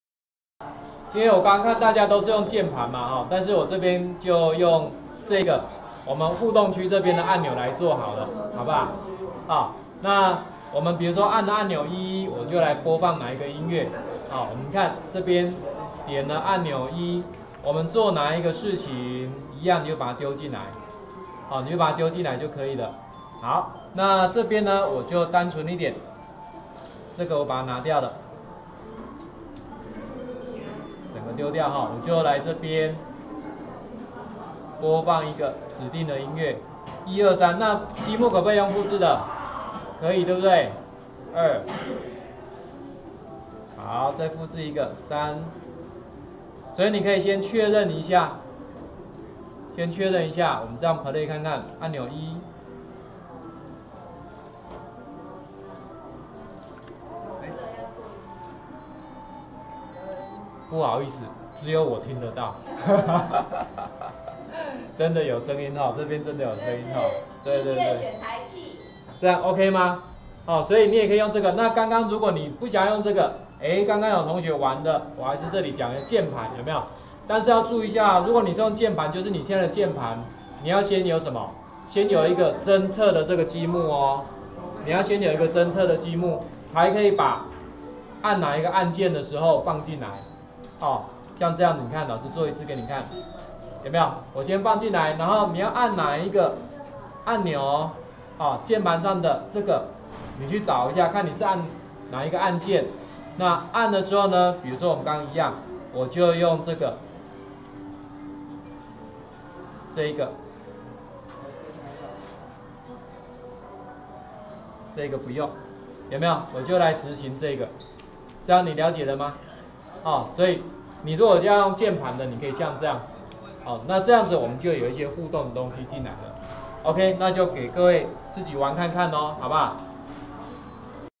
04：蜂鳴器的使用 3：用網頁互動按鈕與鍵盤控制
此研習錄影影片為「20170427 日南國中：Webduino Smart 研習 (3)：Webduino 蜂鳴器的使用、光敏電阻」的研習錄影！